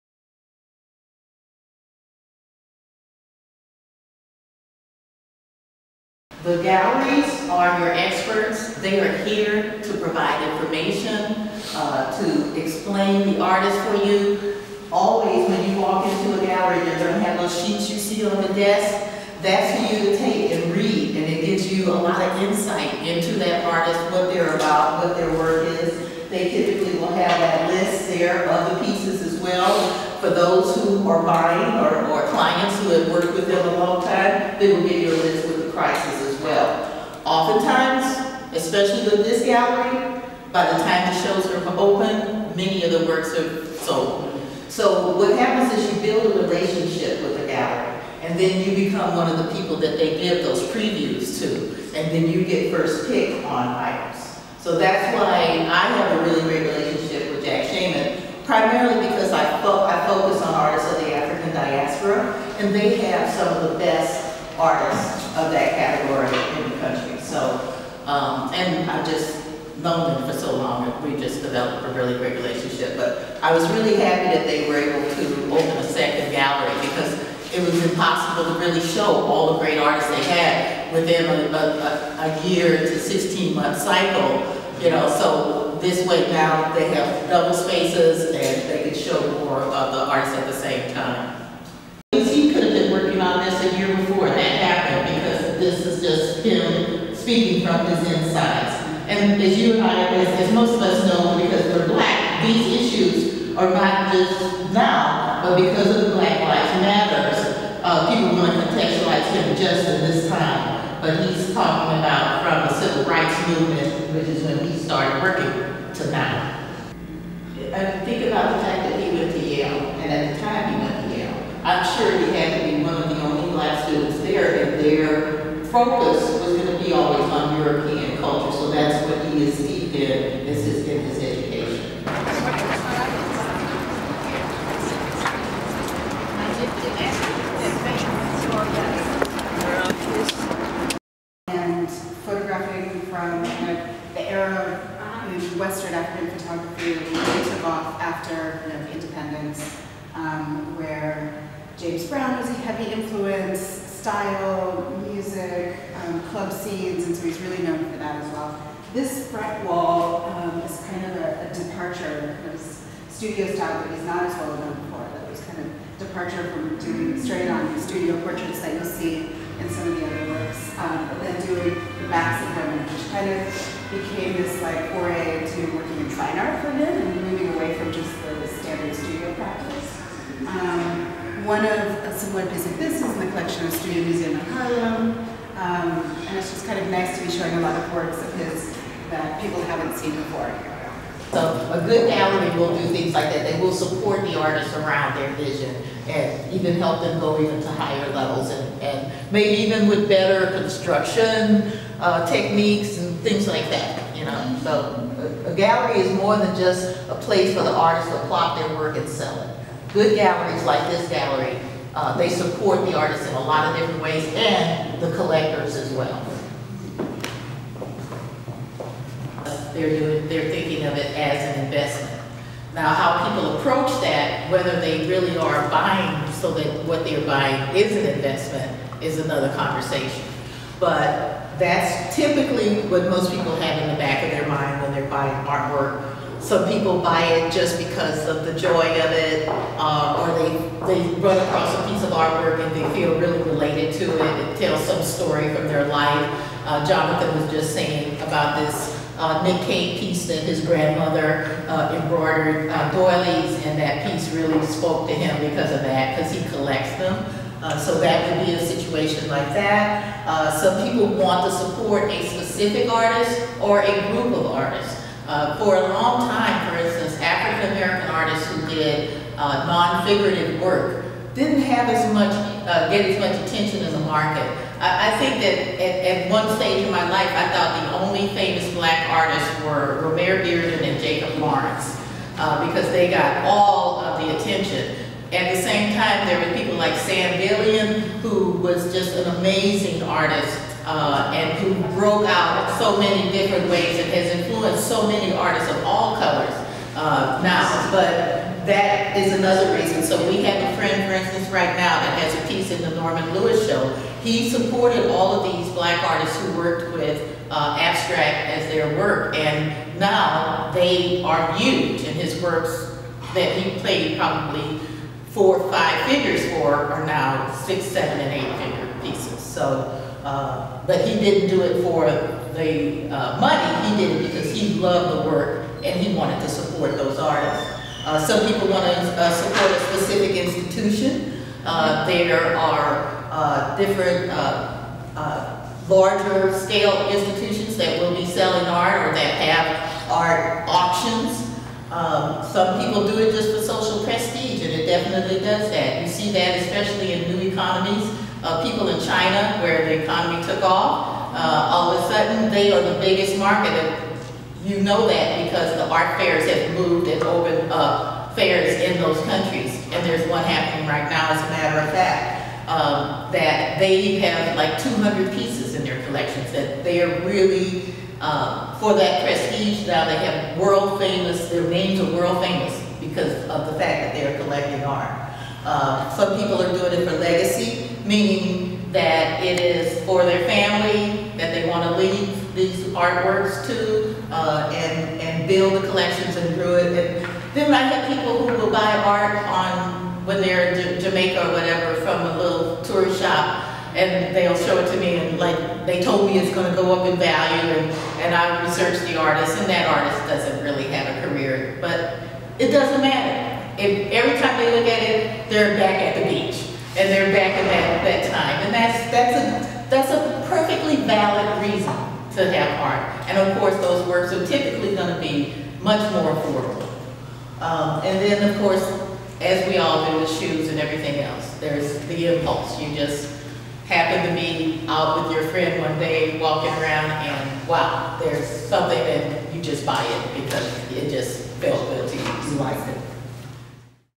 The Art of Art Consulting: Cheryl R Riley
View this video from THE ART OF ART CONSULTING: Walk and talk at Jack Shainman Gallery, NYC, April 2, 2016

At this event Artist and Art Advisor, Cheryl R. Riley, met participants at the 524 West 24th Street, NYC Jack Shainman Gallery location to view and talk about the Barkley L. Hendricks show.  The group then walked 4 shorts blocks to the 513 West 20th gallery where she shared information about the fundamentals of collecting, explain her process of art advising and answered questions.